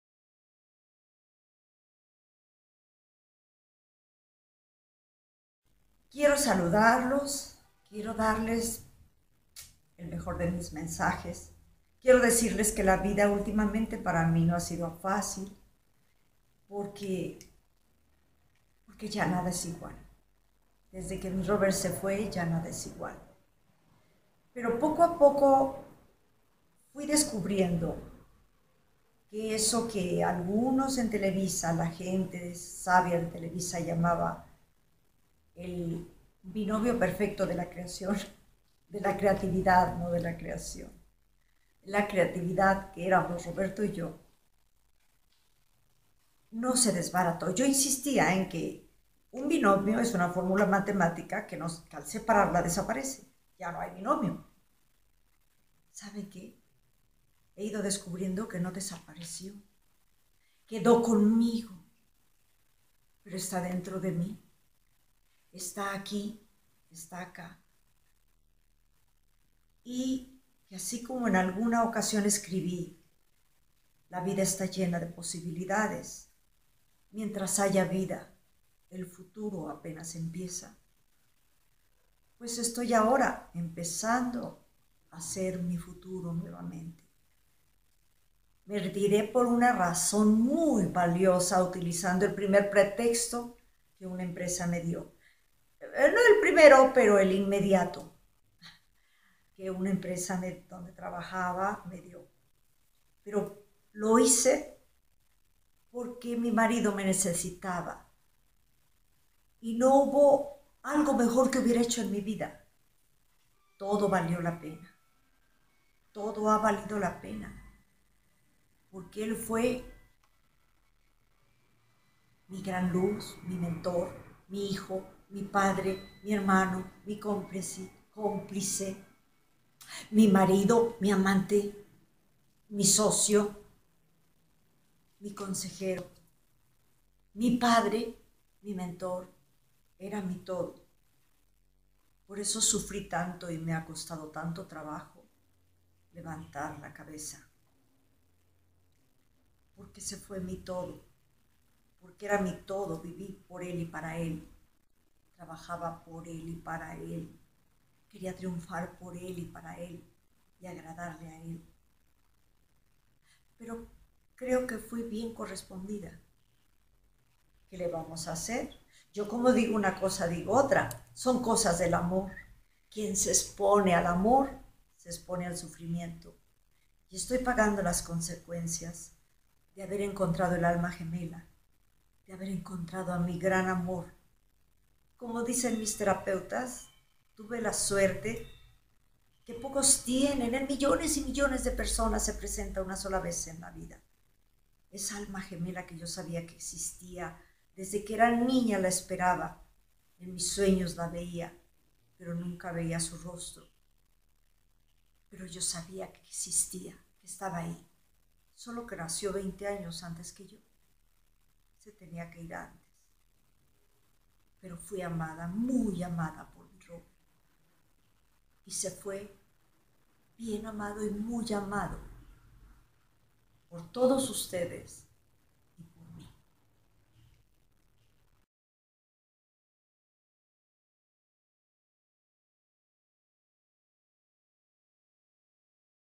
Primer mensaje de Florinda Meza para su canal de Youtube( legendado PT)
Primeira mensagem de Florinda para seu canal do Youtube legendado para o Português.